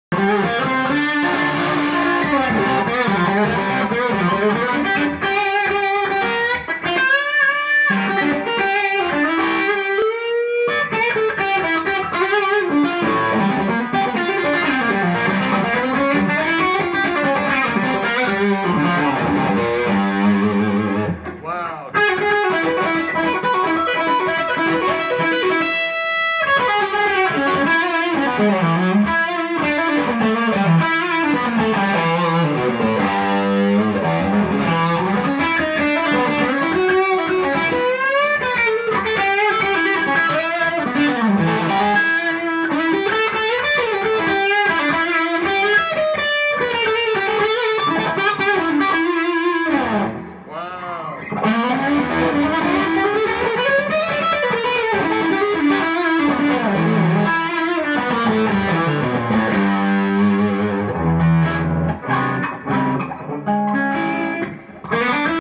1958 ES 335
Dana Olsen playing an Original 1958 Gibson ES 335 thru a mystery amp